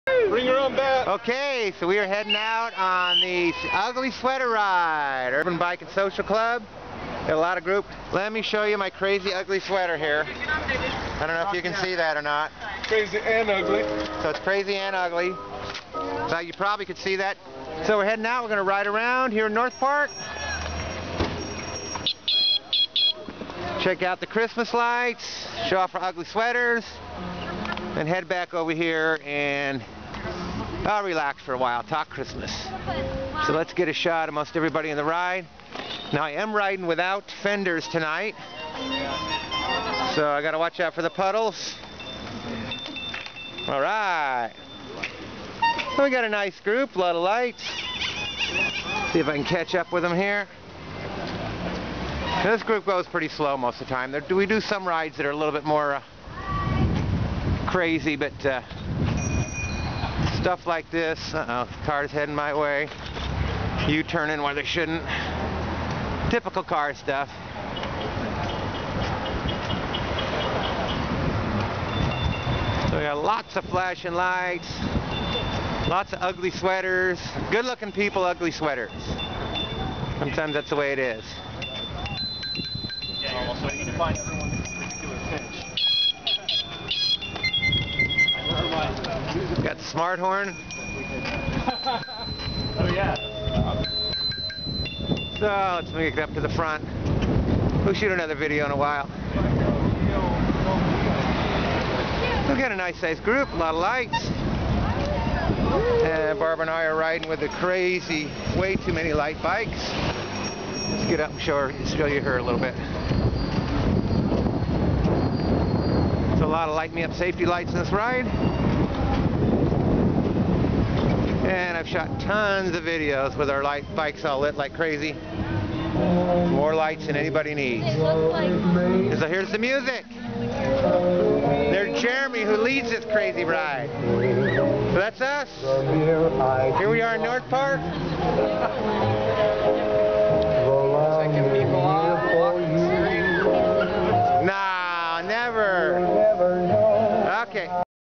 Ugly Sweater Ride Start---Urban Bike & Social Club---2014
With Christmas tunes on the Jeremy's sound system, we left as a group to show off our lights and check out the neighborhood's lights.